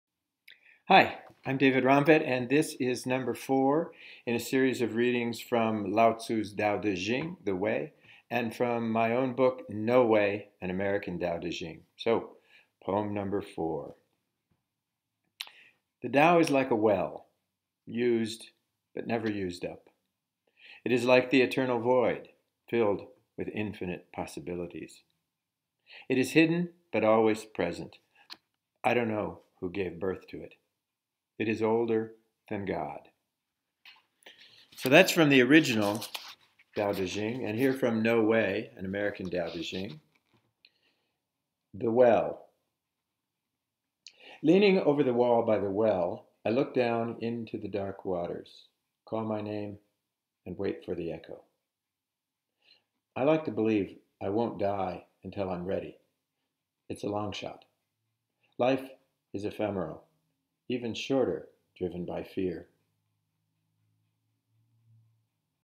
The Well and the Way
This is poem number four in a series of readings by David Romtvedt from The Tao Te Ching and No Way: An American Tao Te Ching.